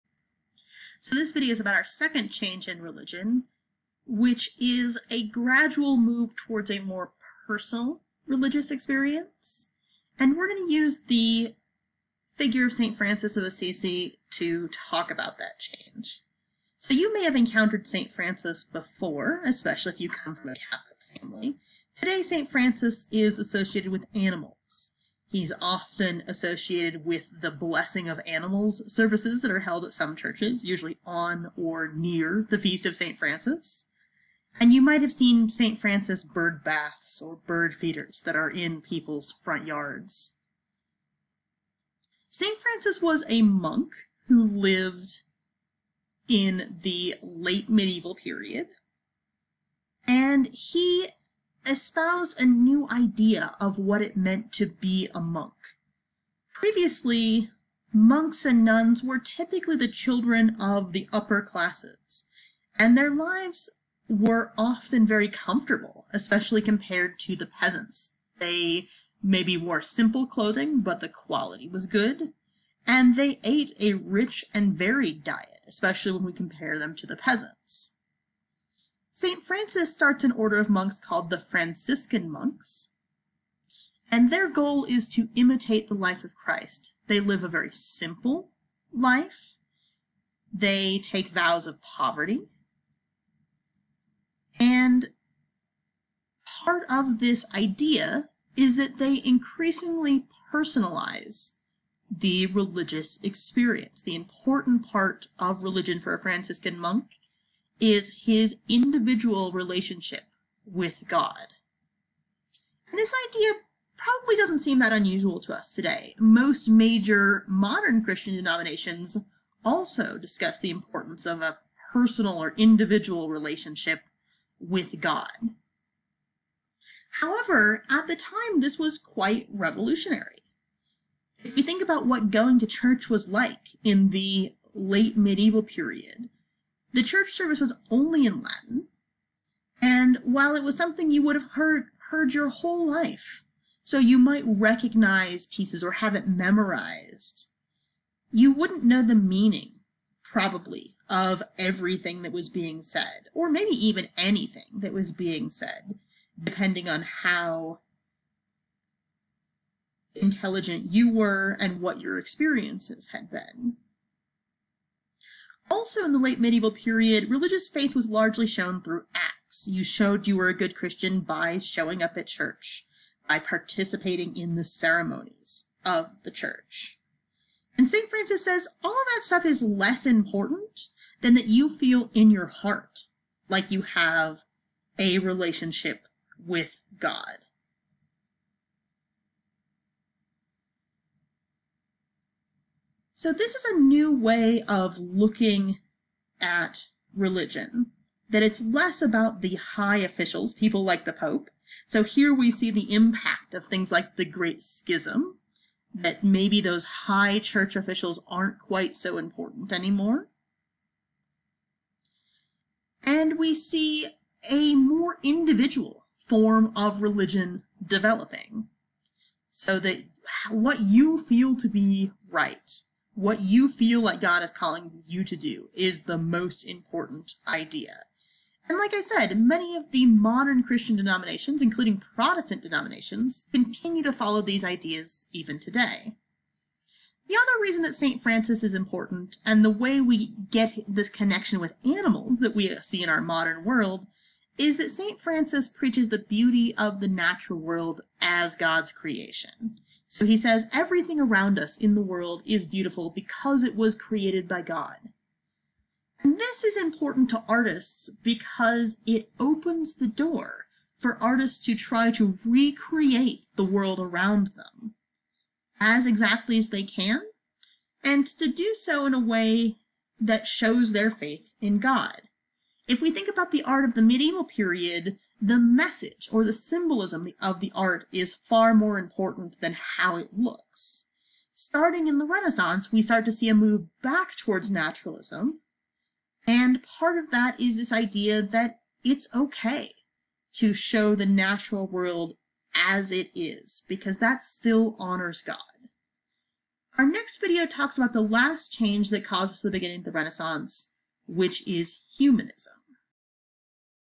Changes from the Med. to the Ren.-St Francis
This video discusses the impact of St. Francis of Assissi on religion and art in the Early Renaissance period